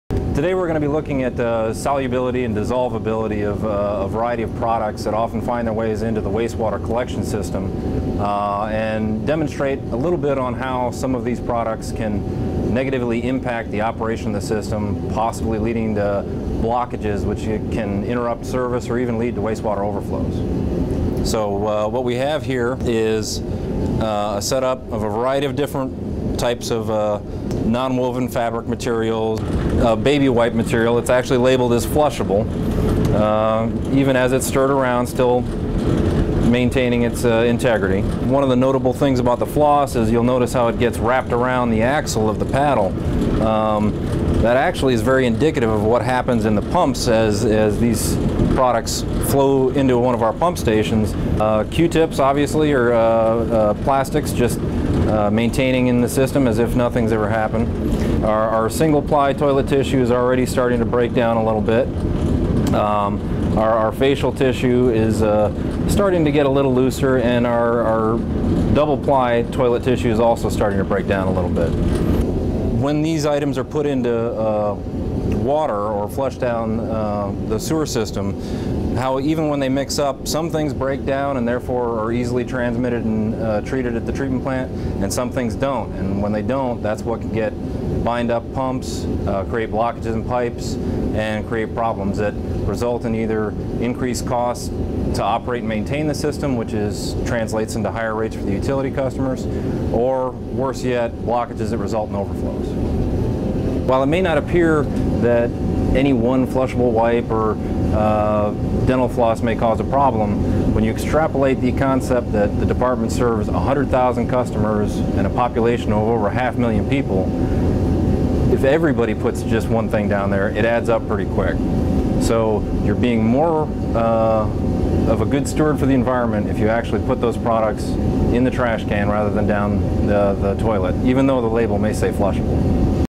City of Tampa Wastewater Department
Tampa Wastewater Director Anthony Kasper show us some items that should never end up in your toilet, and just what sort of damage these items can cause to the sewer system.